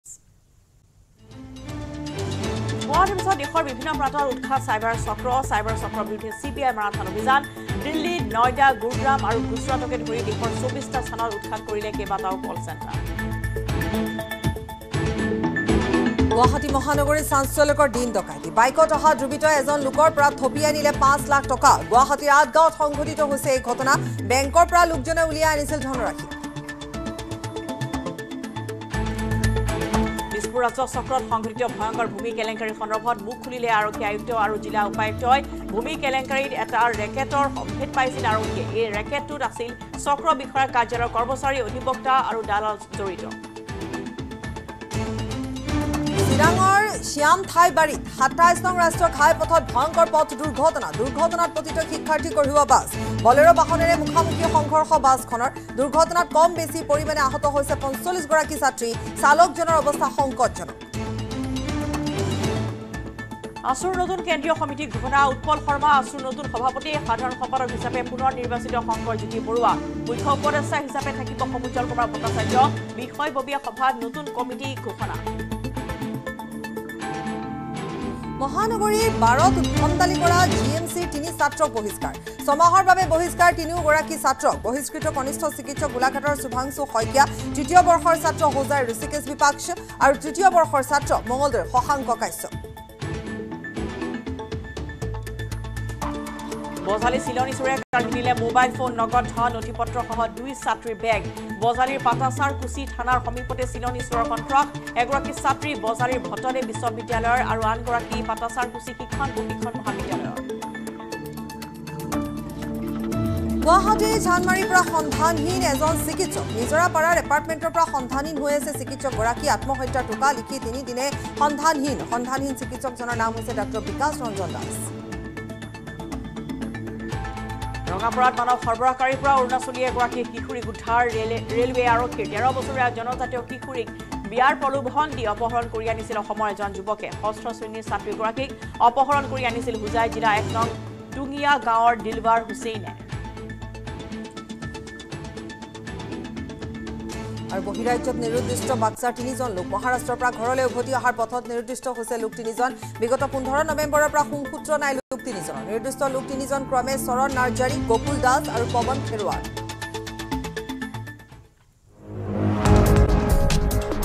TOP 10 HEADLINES : গুৱাহাটীৰ পাছত দেশৰ বিভিন্ন প্ৰান্তত উৎখাত চাইবাৰ চক্ৰ
* গুৱাহাটী মহানগৰীত চাঞ্চল্যকৰ দিন ডকাইতি
* ভূমি কেলেংকাৰীত এটা ৰেকেটৰ সম্ভেদ পাইছিল আৰক্ষীয়ে